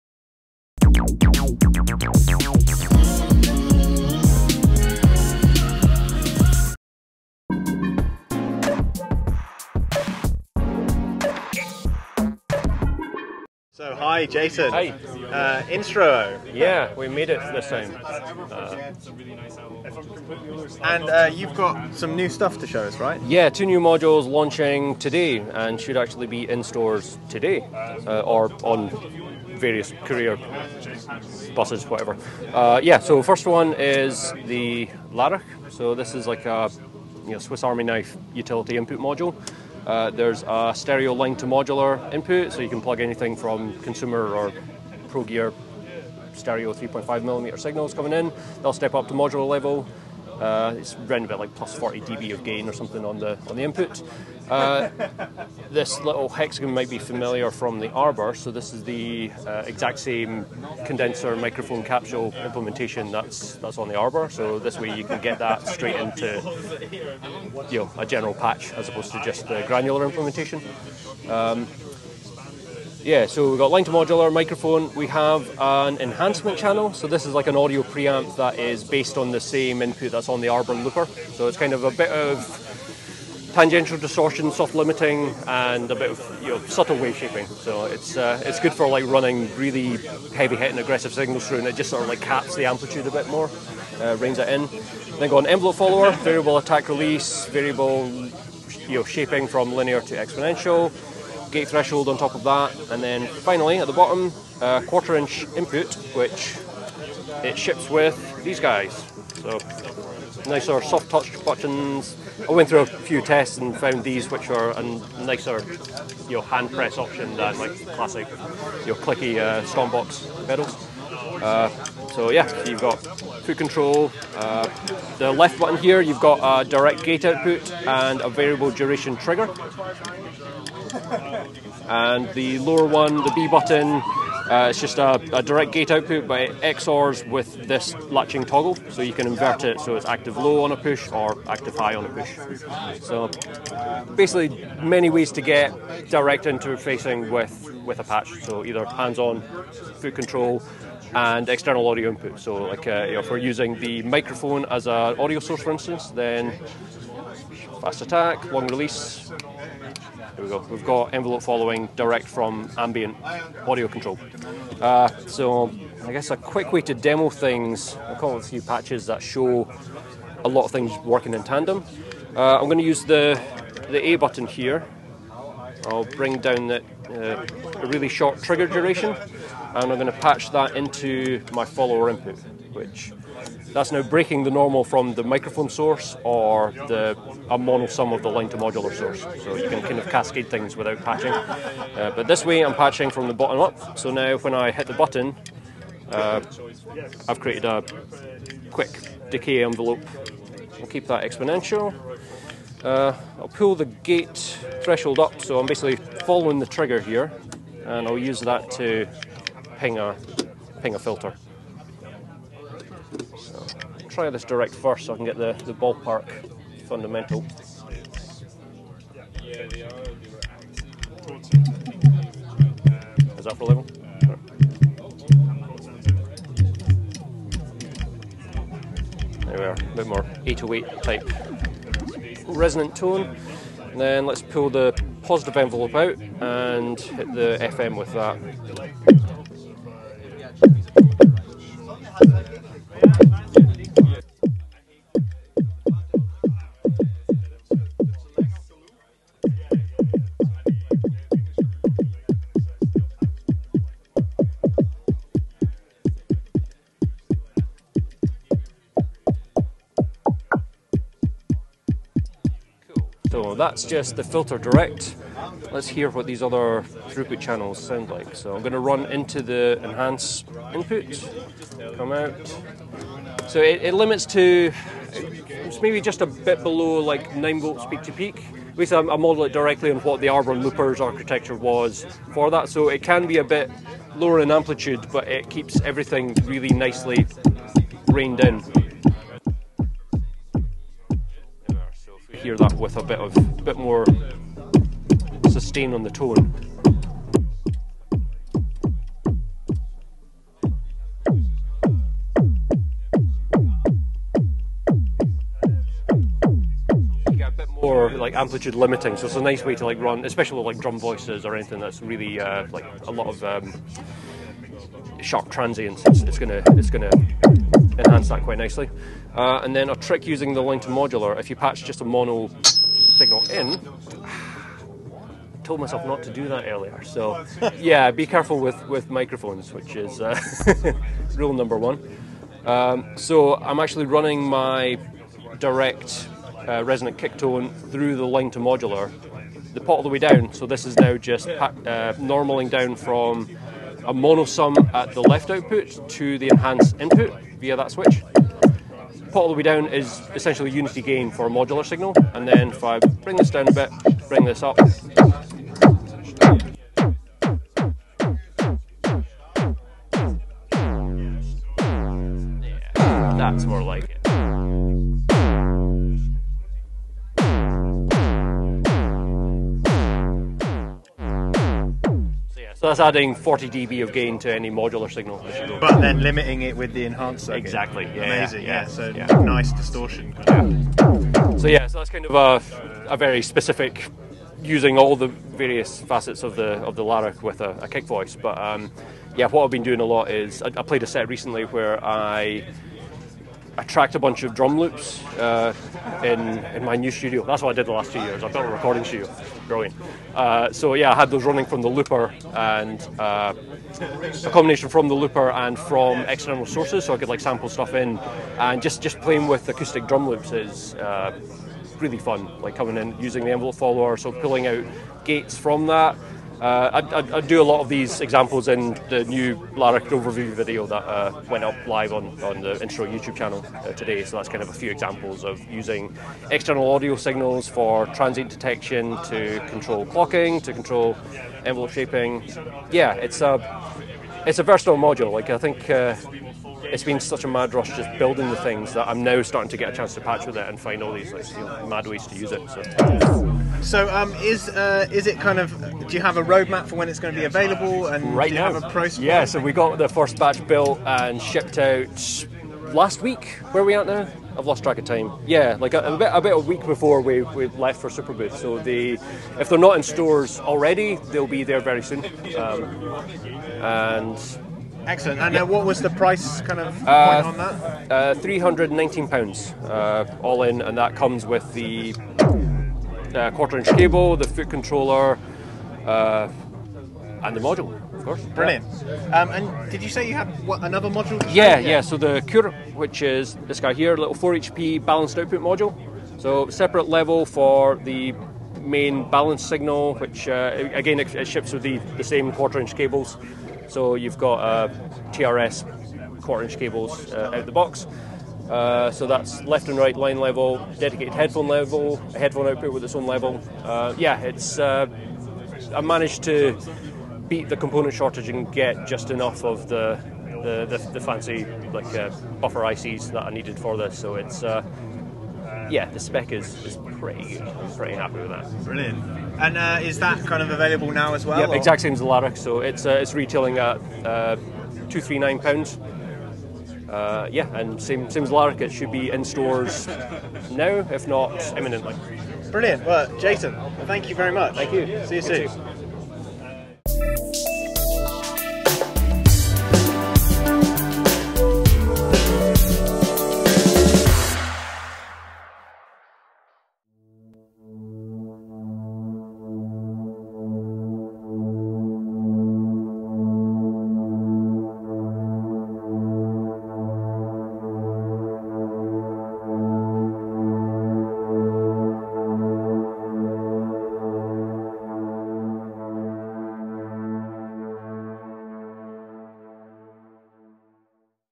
Superbooth 22: Instruõ - Two New Modules
Glasgow-based Instruõ make hand-built quality Eurorack modules. Jason introduced us to two new designs: Làrachd and Cuïr.

Làrachd is a fully analogue multifunction utility module that provides various methods of interfacing external signals and interactions into musically useful control voltage signals. It has an enhancement channel providing distortion and soft-limiting, a built in condenser microphone to generate signals from the ambient environment, and it can be controlled with the included two-button foot switch.

The Instruō Cuïr is a final stage output module designed to interface with professional audio equipment outside of the modular synthesis ecosystem.

Làrachd and Cuïr are available now.

Làrachd Price: £319. Cuïr Price: £239